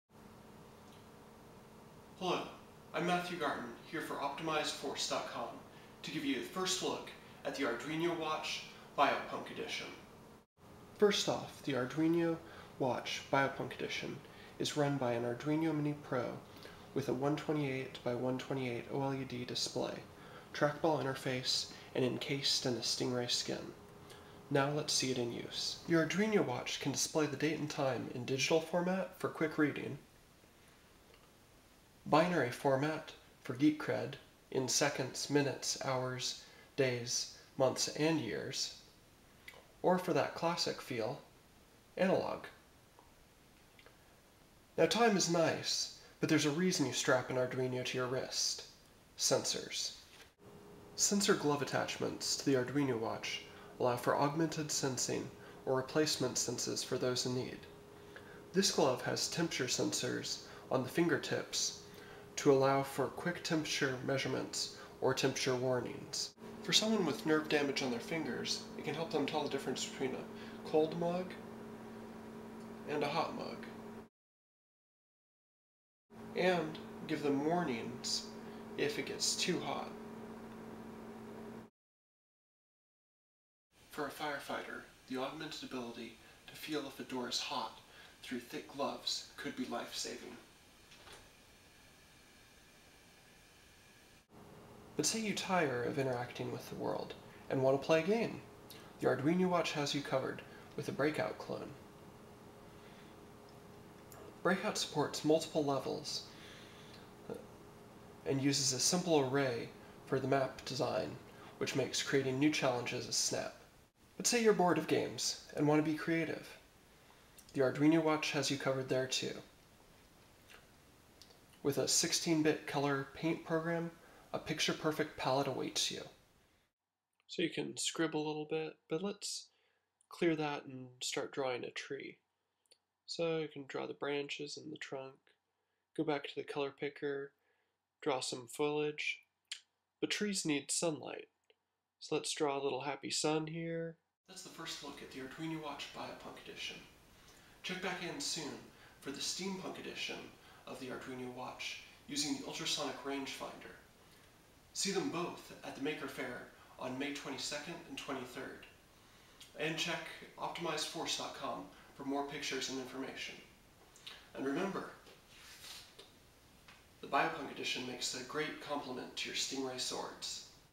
Arduino Watch: Biopunk Edition
The Biopunk and Steampunk Arduino Watches provide augmented sensing of temperature and range, 16-bit color drawing program, Breakout game, and also tells the time in your choice of digital, binary, or analog.  Additional sensors, devices, and programs are easy to add as any standard Arduino.